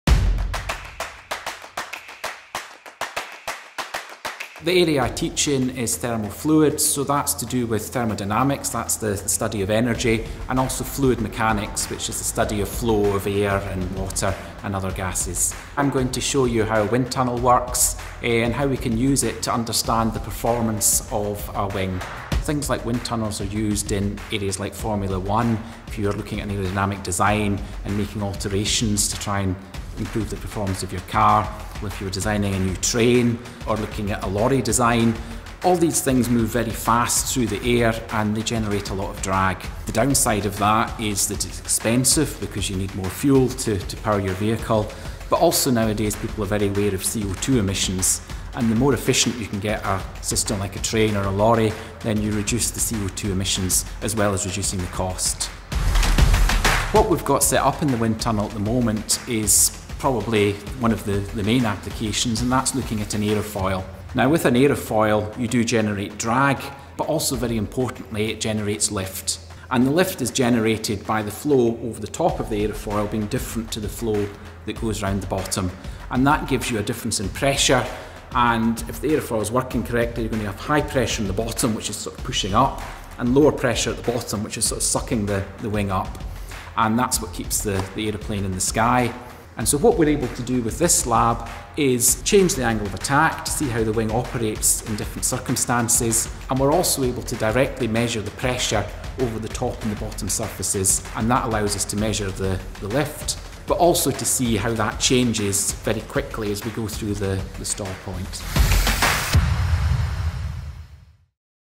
Take a look at our wind tunnel | University of Portsmouth
Wind tunnels can be used to study the aerodynamic design of cars, trains and planes. Dr James Buick shows you how one works.

Learn more about mechanical design and engineering here at uni.port.ac.uk/2PoT3Mr